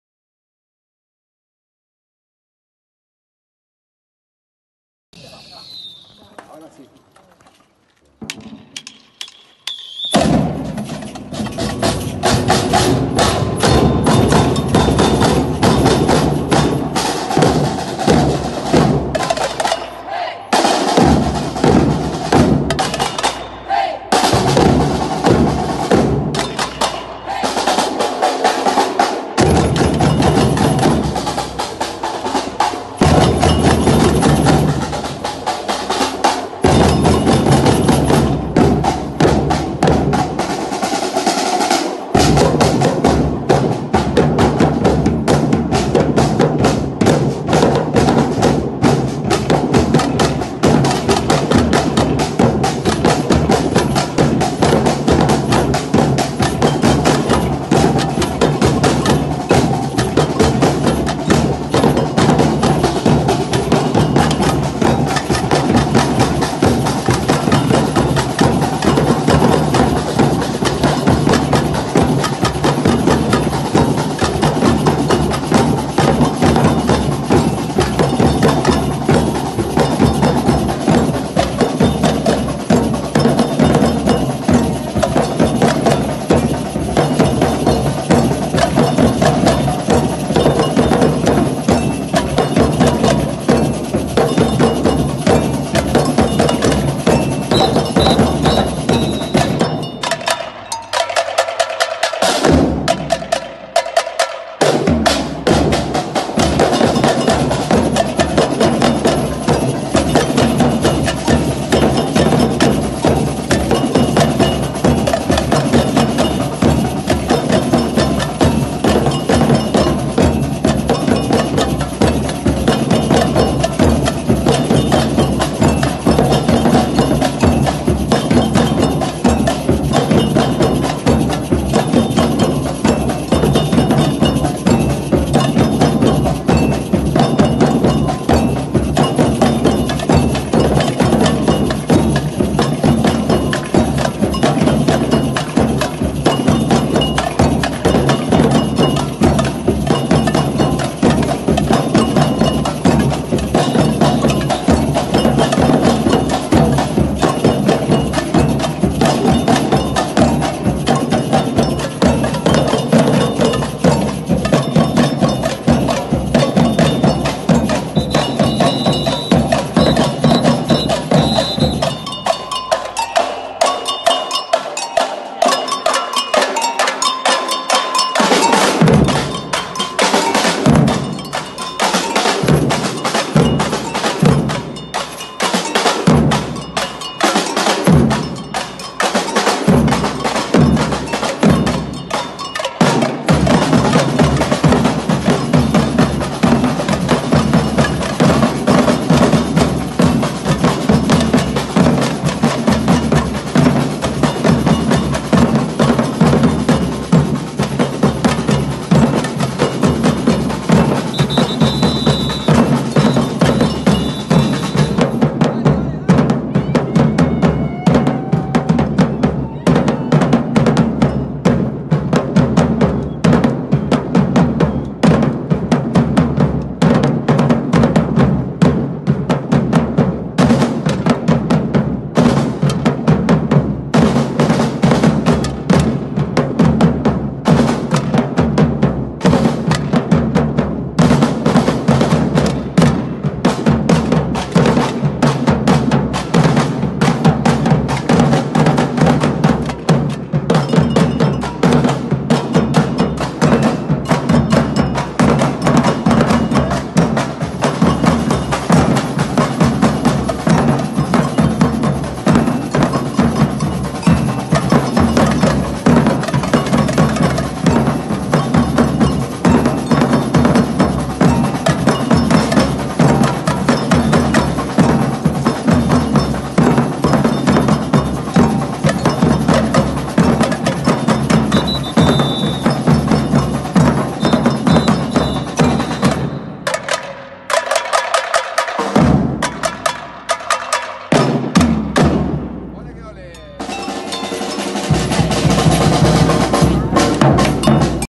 30/01/20 LANZAROTE Batucada por la Paz y la Noviolencia
La Asociación Socio Cultural “Merekumbé” dedica su ensayo del miércoles 29 de enero al “Día Escolar de la NoViolencia Activa y por la Paz” que celebramos el 30 de enero.
En el centro sociocultural del pueblo de Muñique, Lanzarote (provincia de Las Palmas, Islas Canarias, España).
Desde tierra de fuego, insuflamos aliento con este ritmo a fuerza de batucada y la mejor buena onda desde la Pachamama. Por la Paz y la concordia del Espíritu mundial y hoy, en especial en la Escuela. Por el respeto entre nuestros semejantes y entre culturas, celebremos!